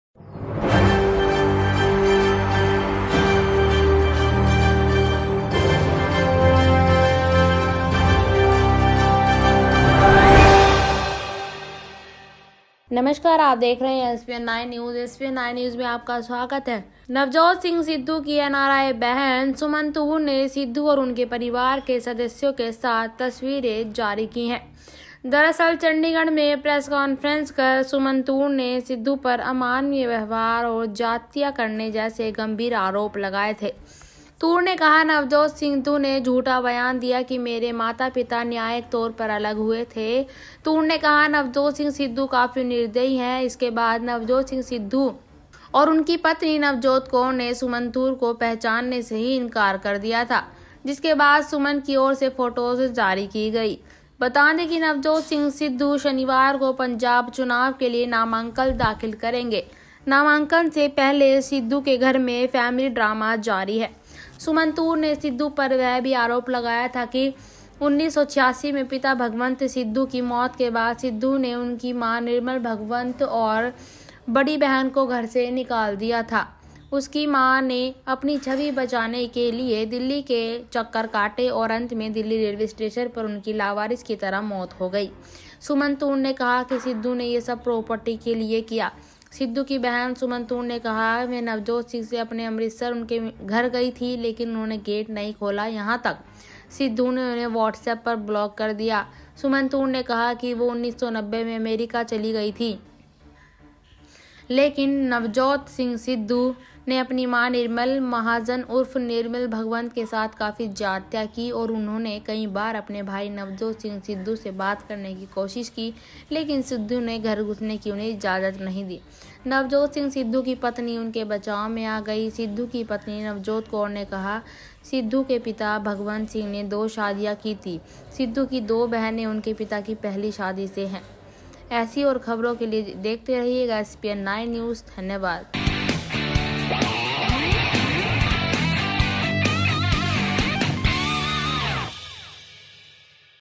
नवजोत सिंह सिद्धू की बहन ने दिखाई एक फोटो और इस चीज़ पर किया बड़ा खुलासा
सिद्धू की बहन सुमन तूर ने प्रेस कॉन्फ्रेंस कर नवजोत सिंह सिद्धू पर आरोप लगाया कि उनके पिता भगवंत सिद्धू की मौत के बाद नवजोत ने उनकी मां निर्मल भगवंत बड़ी बहन को घर से बाहर निकाल दिया था. इतना ही नहीं उन्होंने कहा, नवजोत सिंह सिद्धू काफी निर्दयी हैं |